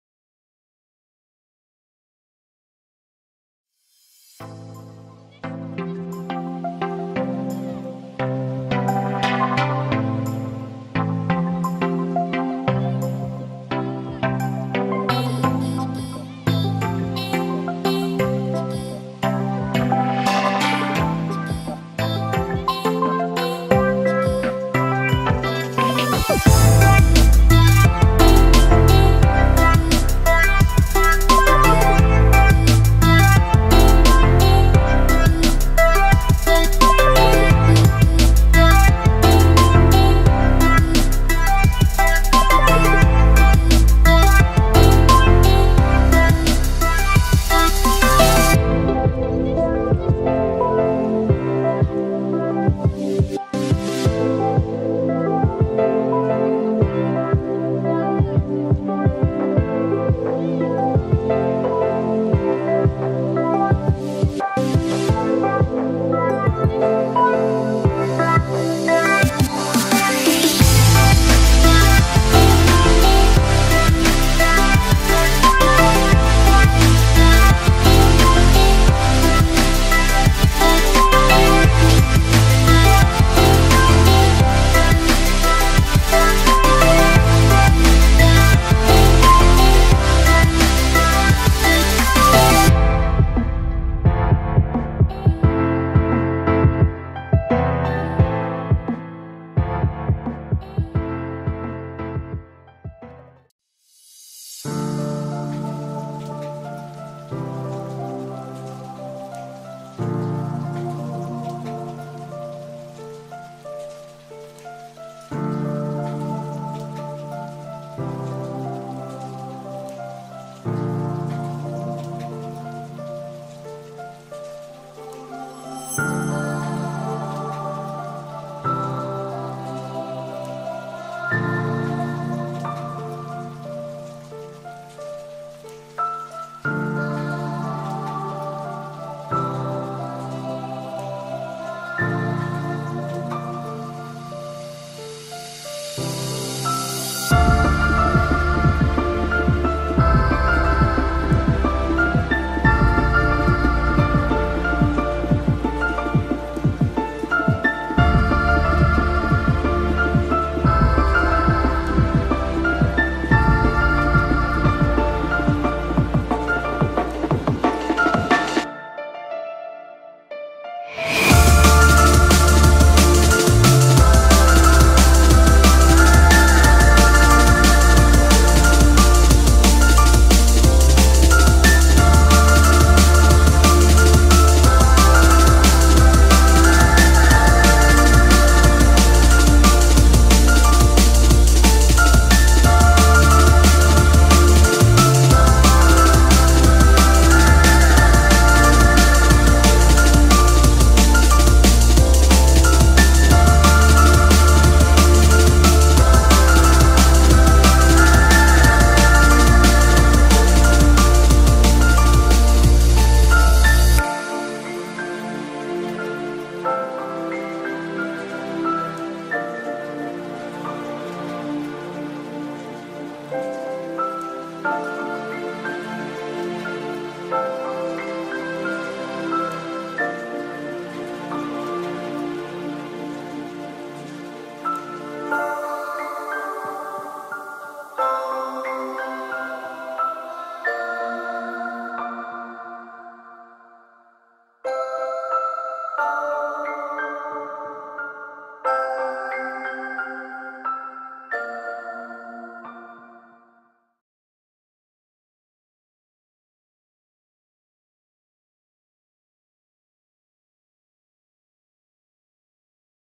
Whistler, Canada Drone Footage (w/music)
Finally found a good editor. Filmed in 4K at Alta Lake, Whistler. So yeah.
Both found on youtube.